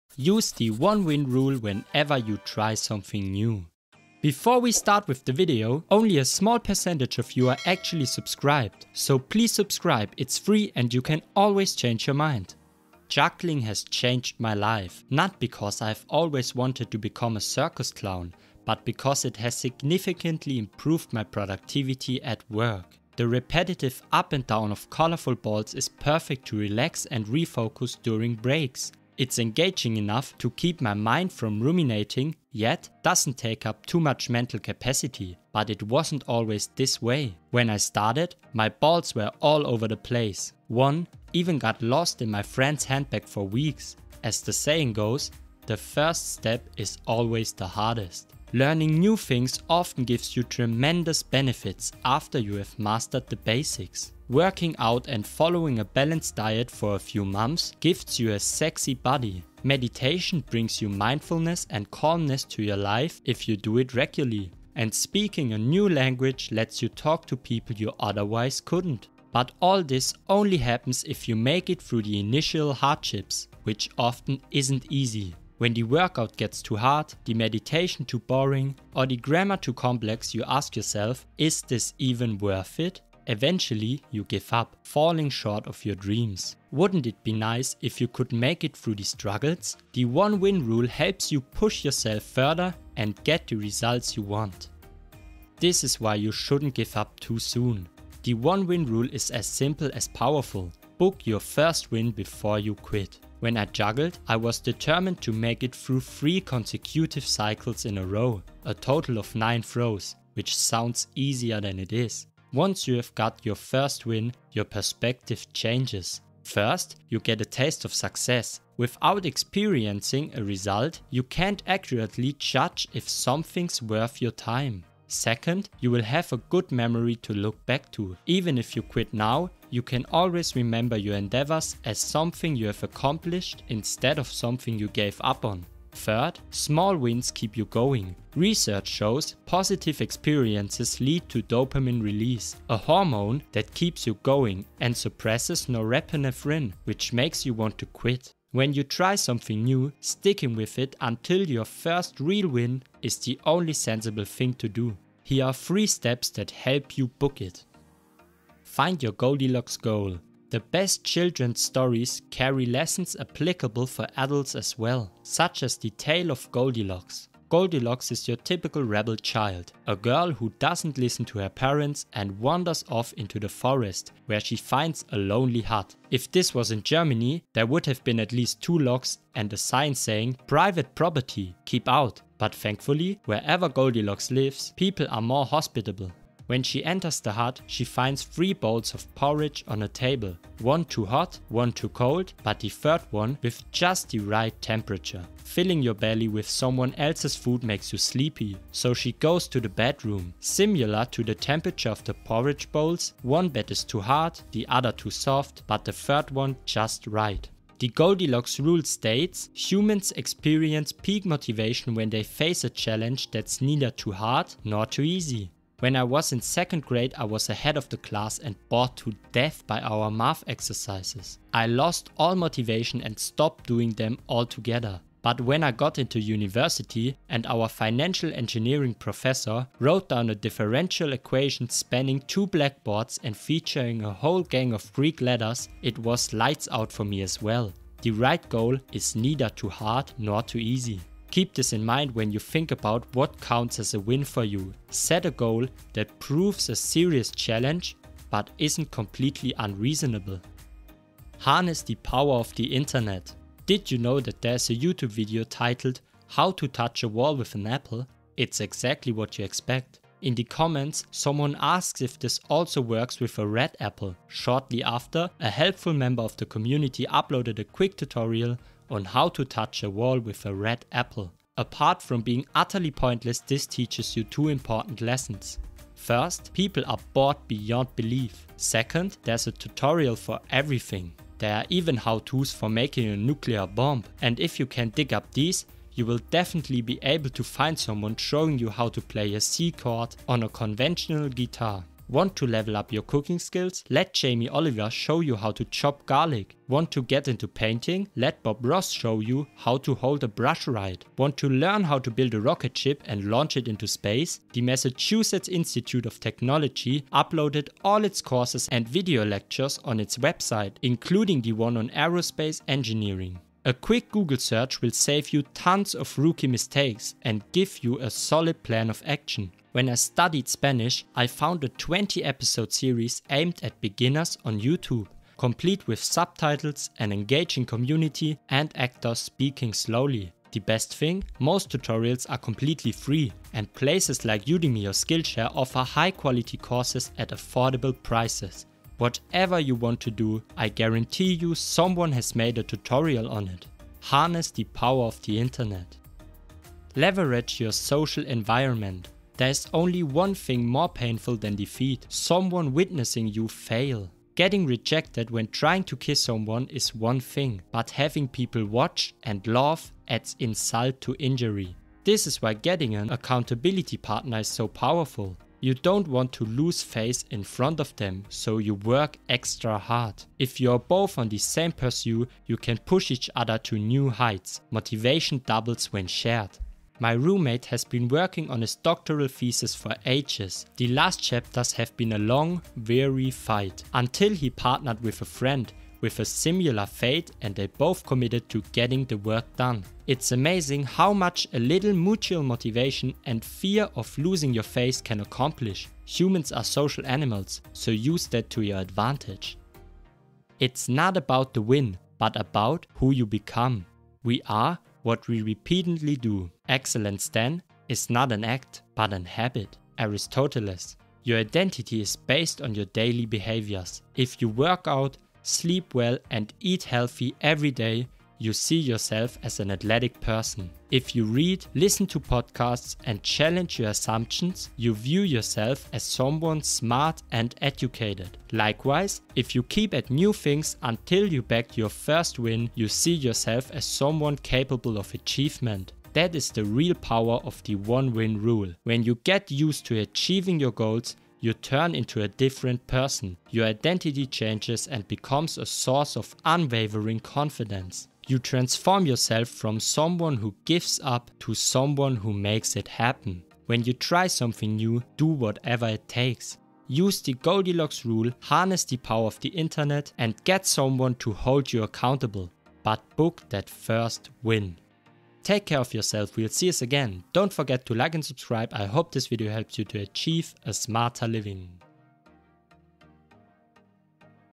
Watch This Before You Give Up! - The 1-Win Rule
Learning new things often gives you tremendous benefits — after you’ve mastered the basics. But this only happens if you make it through the initial hardships, which often isn’t easy.

⏱| Timestamps:
0:00 The Initial Hardships
1:35 Don't Give Up Too Soon
2:37 How To Win - Step 1
4:11 How To Win - Step 2
5:47 How To Win - Step 3
6:40 It's Not About The Win...

✍️| Author: Moreno Zugaro